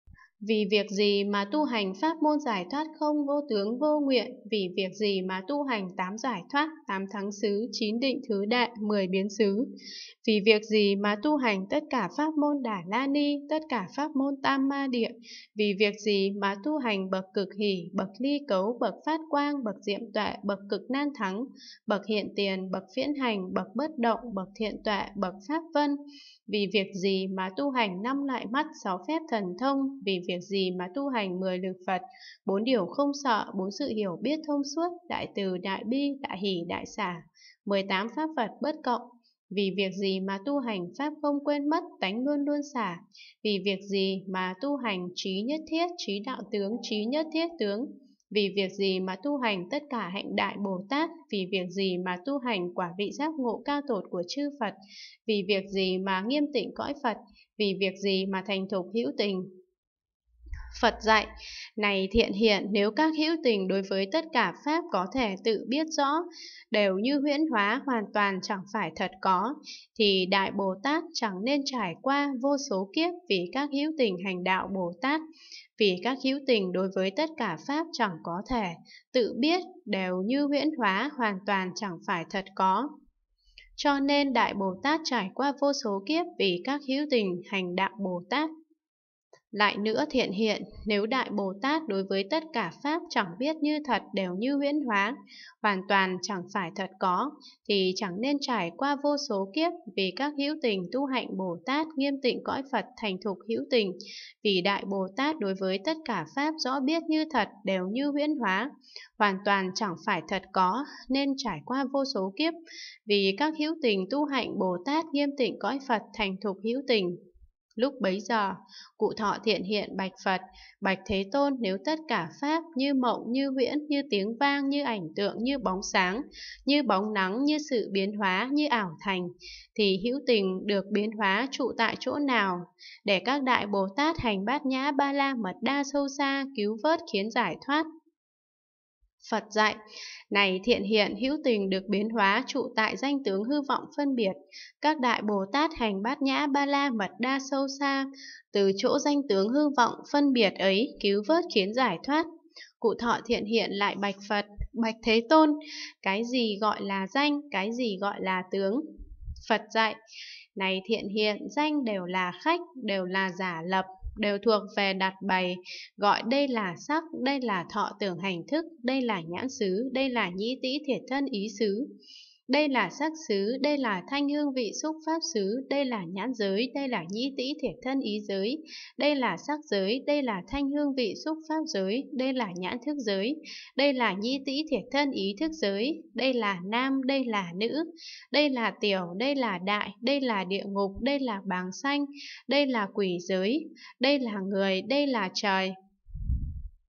Quyển 384: Kinh đại bát nhã ba la mật đa - Phẩm các pháp bình đẳng (rất hay) (384/600) (2)
Phẩm 69: Phẩm các pháp bình đẳng
Hán dịch: Pháp sư Huyền Trang
Việt dịch: Hoà thượng Trí Nghiêm
Giảo chính: Hoà thượng Quảng Độ